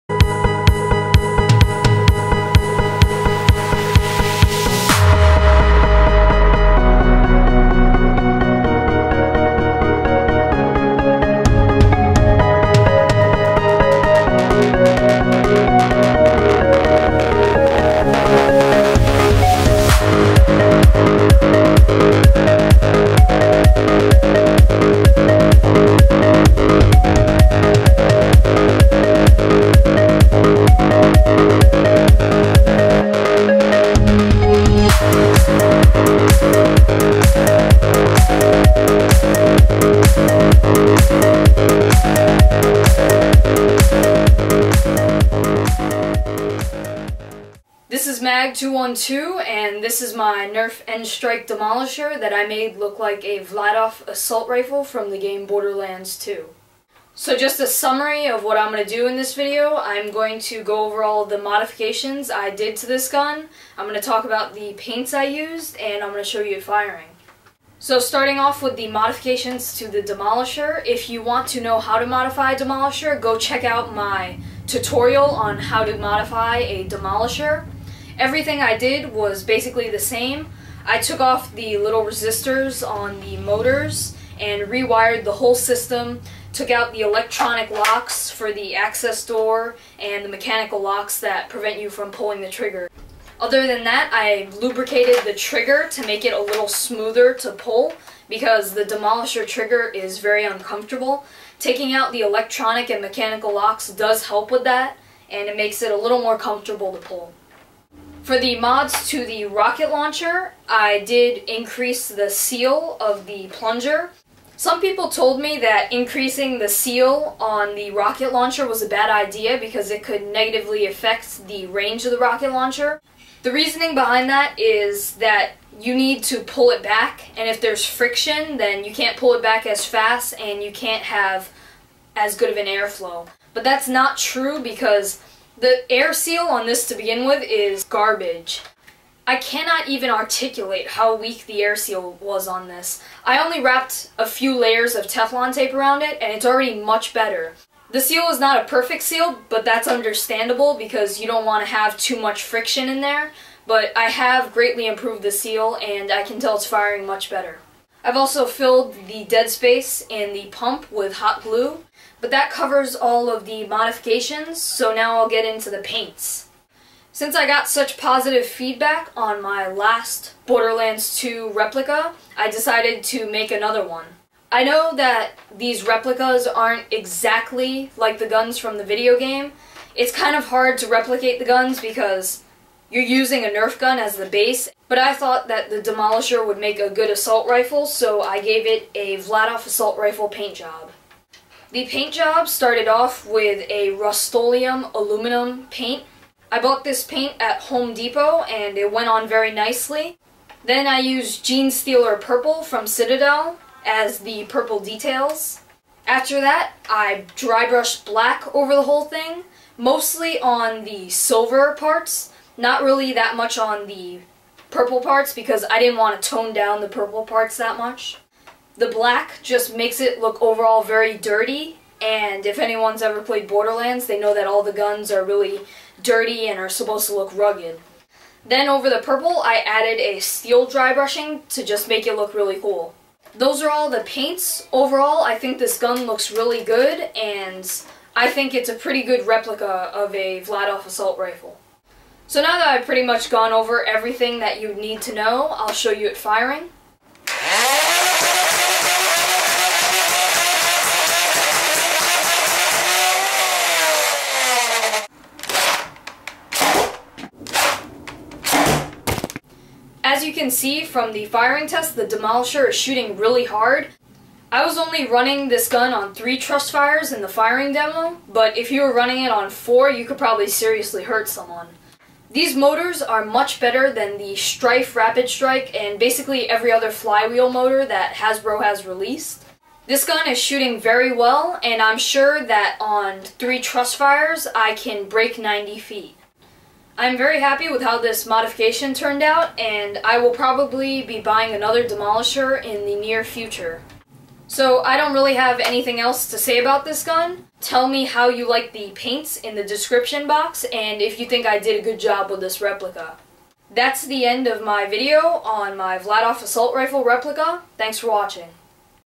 [MOD] Borderlands 2 Vladof Assault Rifle [Modified Nerf Demolisher 2 in 1]
This gun isn't an exact replica but nonetheless this is my vladof assault rifle replica with the Demolisher as the base.